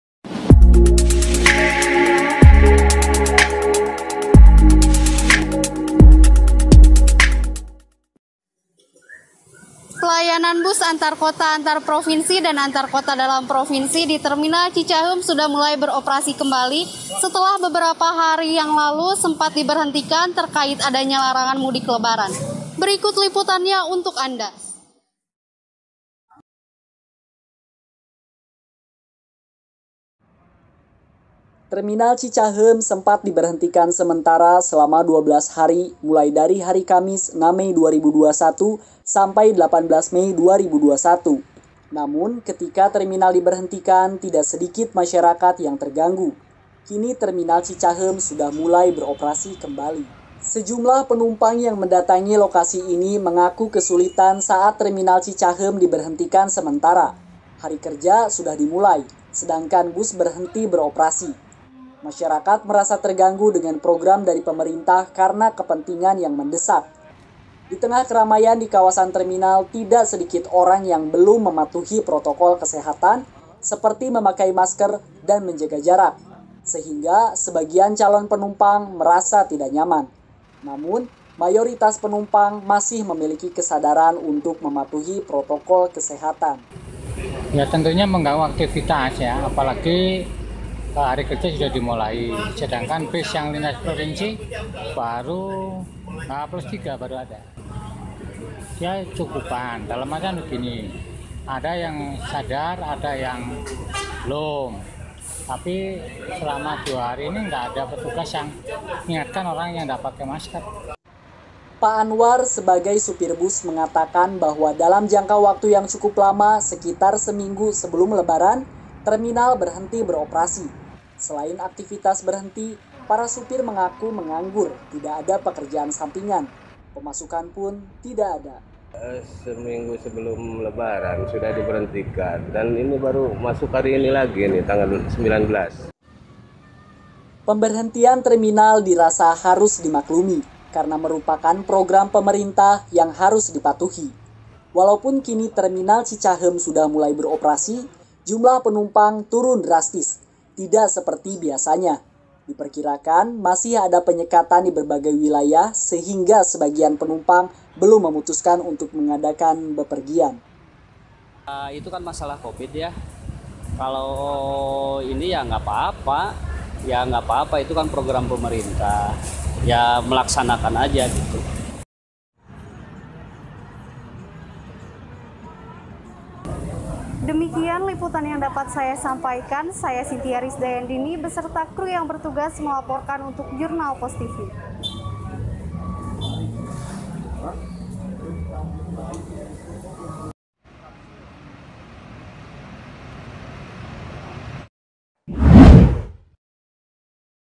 Terminal Cicaheum Sudah Mulai Beroperasi Kembali | NEWS
KARYA KELOMPOK OPREC JURNALPOSMEDIA KELOMPOK (12) DIVISI TV

Pelayanan bus antar kota antar provinsi (AKAP)  dan antar kota dalam provinsi (AKDP)  di terminal Cicaheum sudah mulai beroperasi kembali, setelah beberapa hari sempat diberhentikan beroperasi terkait larangan mudik lebaran.
Pemimpin Umum
Ahmad Baihaqi

Pemimpin Redaksi
Intan Riskina Ichsan

Redaktur TV
M. Rendhy Wibowo

Koordinator Liputan
Zidan Raihan Firdaus

Reporter Data
Anisa Fitriani

Reporter On Cam
Chyntia Risdayandini

Dubber
Tegar Setiadi

Campers
Lidya Dwi

Editor
Alivia Fairisya Roshiani